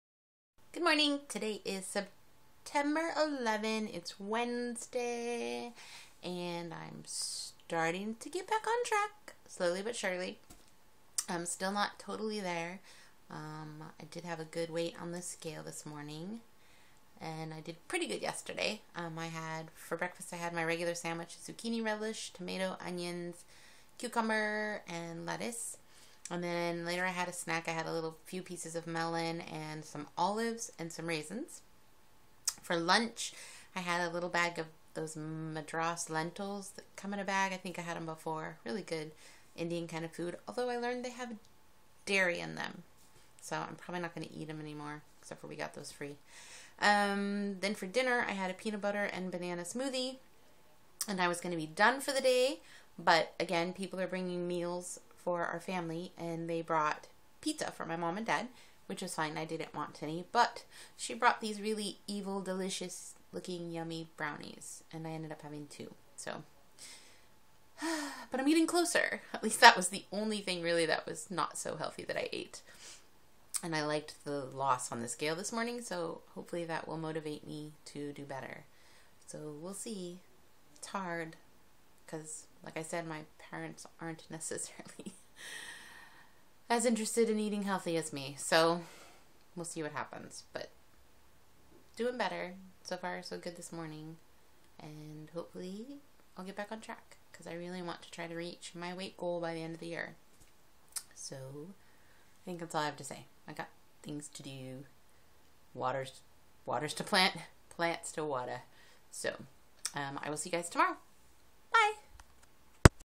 Day 73 - Evil Brownies
September 10 meals -
Breakfast - zucchini relish, tomato, onion, cucumber and lettuce sandwich
Snack - raisins, melon and 3 olives
Lunch - Madras Lentils
Dinner - peanut butter and banana smoothie
Snack - evil brownies :)